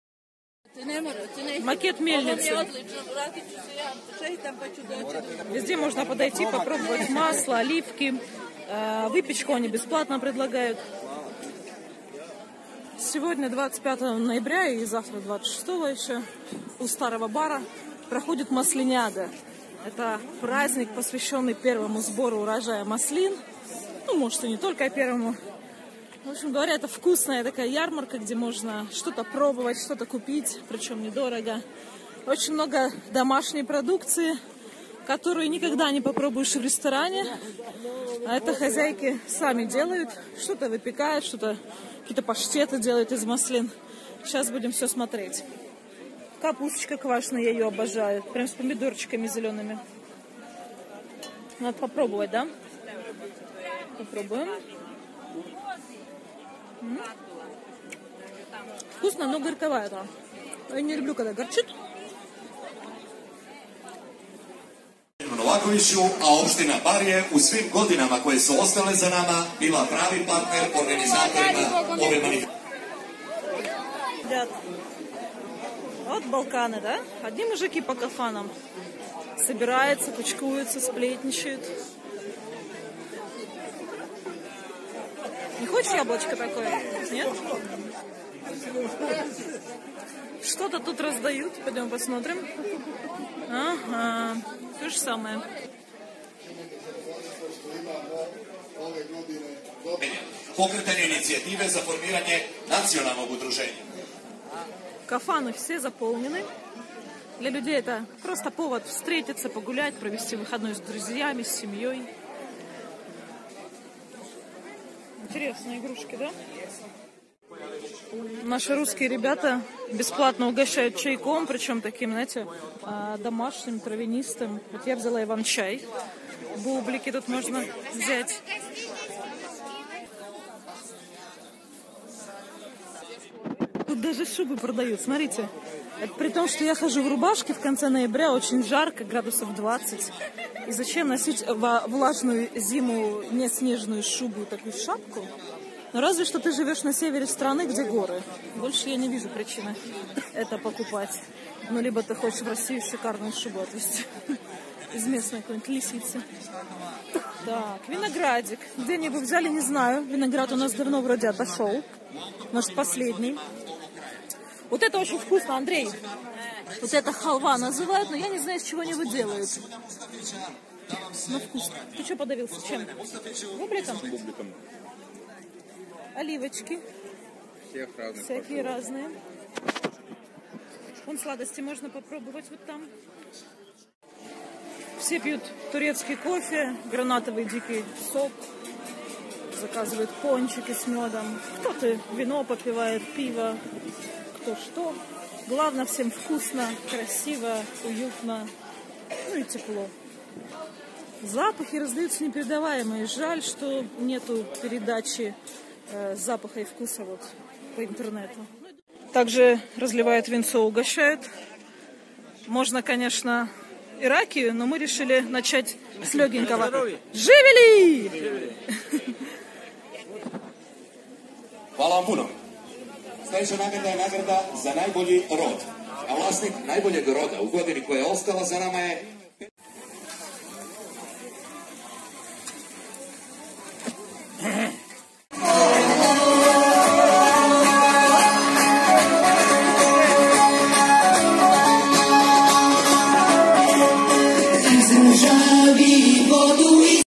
Маслиниада 2017 в Баре - праздник сбора урожая маслин в Черногории
Один из самых вкусных праздников в Черногории я считаю Маслиниаду, которая ежегодно в ноябре проходит в старом Баре. Подробнее читайте на сайте
Собрались в отпуск в Черногорию?